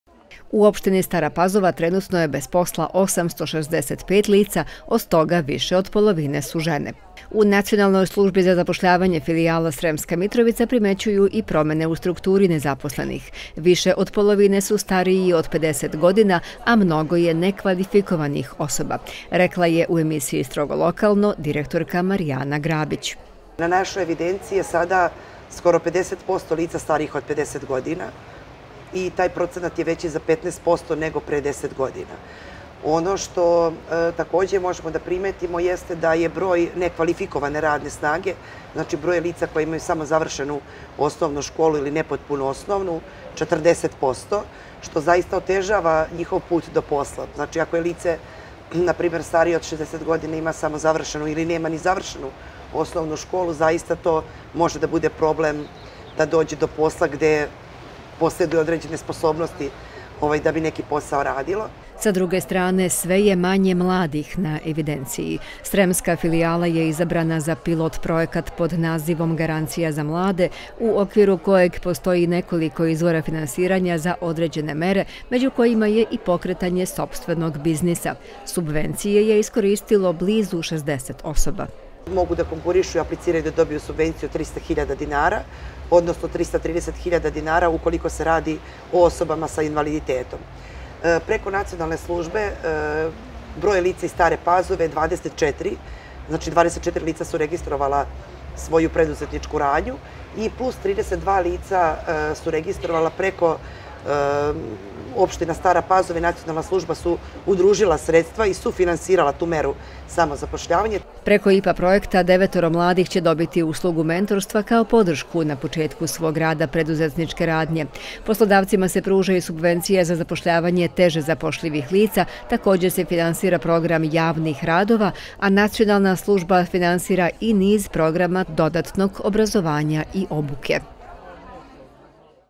Velike šanse za zapošljavanje mladih
Posmatrajući evidenciju osoba koje su trenutno bez posla, u Nacionalnoj službi za zapošljavanje, filijala Sremska Mitrovica, veliki je broj starijih od 50 godina i nekvalifikovanih a sa druge strane, mladi imaju dosta mogućnosti za zapošljavanje i pokretanje sopstvenog biznisa.